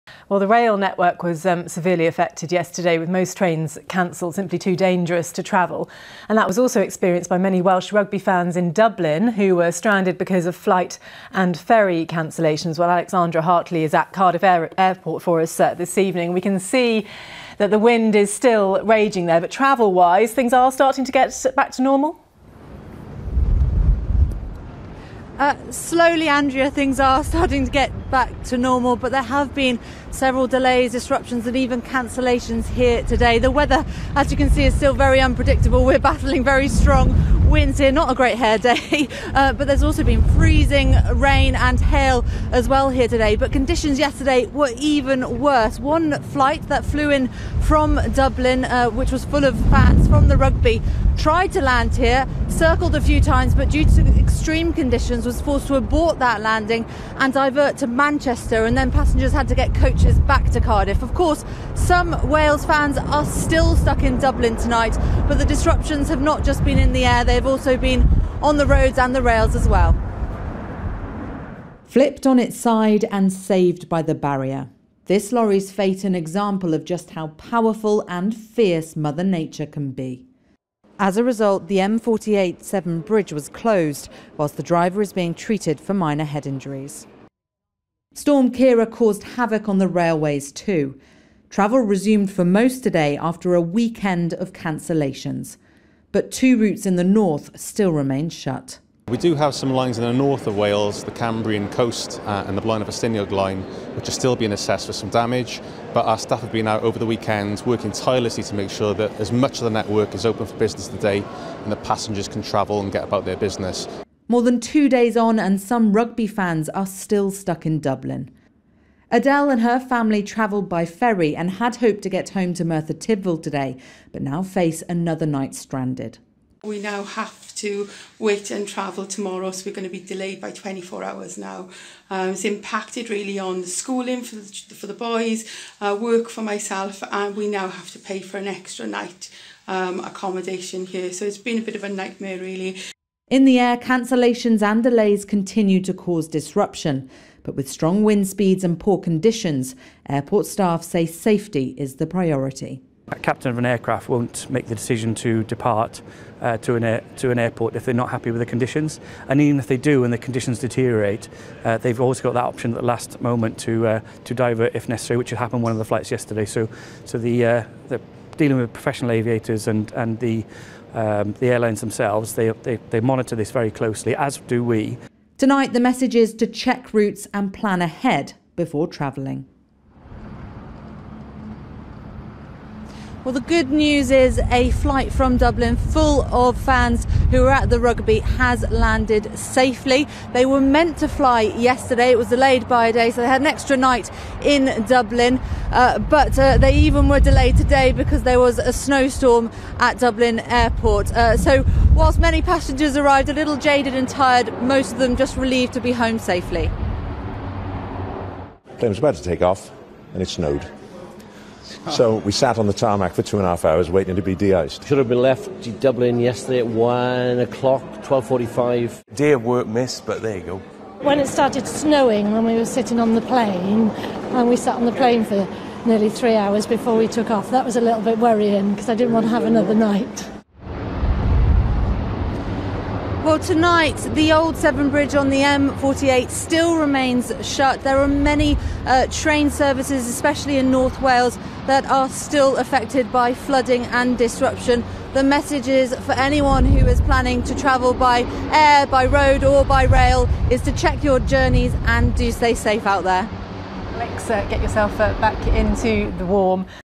Storm Ciara causes chaos in the air, roads and rail
Transport for Wales faces a list of repairs from the damage caused by Storms Ciara. It's not just the railways affected, roads have also been closed.  The M48 Severn Bridge was closed after high winds over turned a lorry.  And Alexandra Hartley is live outside Cardiff Airport where many flights have also been disrupted.